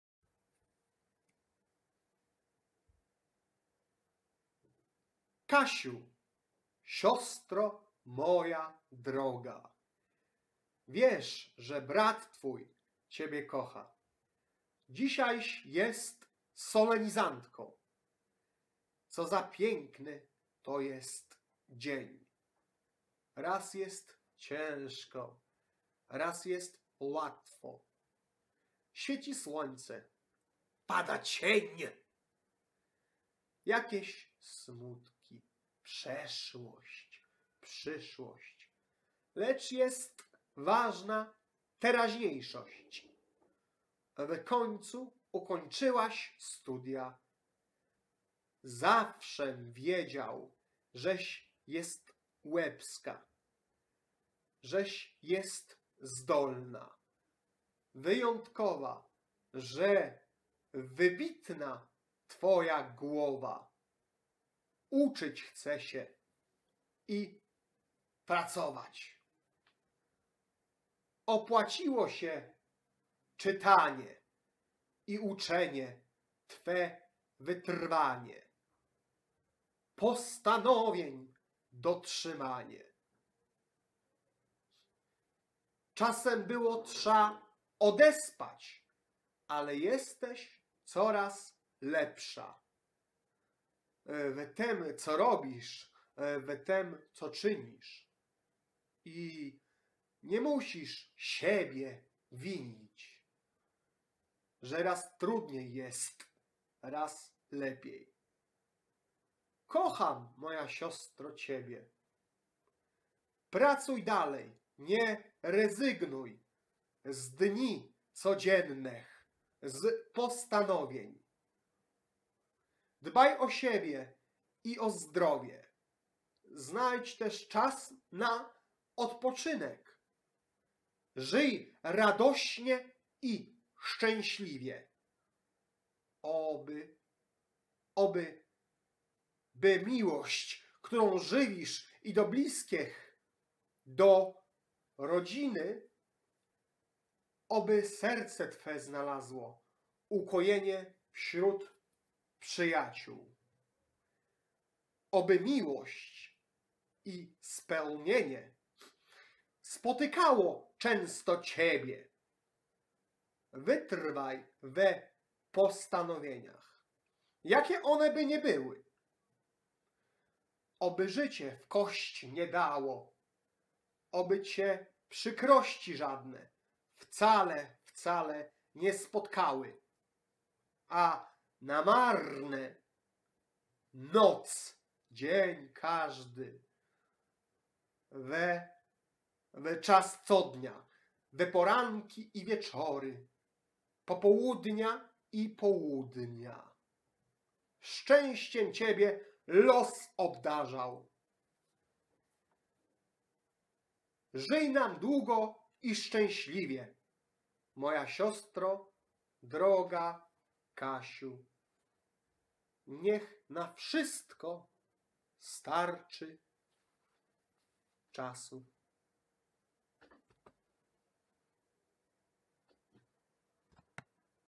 ..VVierszyk dla KASIATORa , ' czyli mojej Siostrze - SOLENIZANTCE !! .. - Sevver
Kasiu - Siostro moja Droga -

Wiesz , że brat Twój Ciebie kocha

Dzisiajś jest Solenizantką -

Co za PIĘKNY to jest dzień

Raz jest ciężko - raz jest łatwo

Świeci Słońce , pada cień

Jakieś smutki - przeszłość przyszłość

Lecz jest WAŻNA teraźniejszość .

W końcu ukończyłaś Studia -

Zawszem wiedział , żeś jest Łebska -

- żeś Jest Zdolna , Wyjątkowa

Że Wybitna Twoja GŁOWA

- uczyć chce się i pracować

Opłaciło się czytanie

i uczenie - Twe wytrwanie

Postanowień dotrzymanie

Czasem było trza odespać -

- ale jesteś co-raz Lepsza

- w tem , co robisz -

- w tem , co czynisz ,

- i nie musisz Siebie winić ,

Że raz Trudniej jest - raz Lepiej

Kocham moja Siostro - CIEBIE -

Pracuj dalej , nie rezygnuj -

- z dni codziennech -

- z postanowień -

Dbaj o Siebie i o Zdrowie

Znajdź też Czas na odpoczynek

Żyj Radośnie i SZCZĘŚLIWIE

Oby - oby - by MIŁOŚĆ

Którą żywisz

i do Bliskiech -

- do Rodziny

Oby Serce Twe znalazło

Ukojenie wśród Przyjaciół

Oby MIŁOŚĆ .. - i SPEŁNIENIE

Spotykało często Ciebie

Wytrwaj we postanowieniach

- jakie One by nie były ! ! !

Oby życie w kość nie dało

Oby Cię przykrości żadne

Wcale

Wcale

Nie spotkały

- a na marne

Noc

dzień każdy

We - w - -czas co-dnia

we poranki

i wieczory

popołudnia

i południa

SZCZĘŚCIEM Ciebie LOS obdarzał

Żyj nam Długo i Szczęśliwie

Moja - Siostro Droga - KASIU -

niech na WSZYSTKO starczy Czasu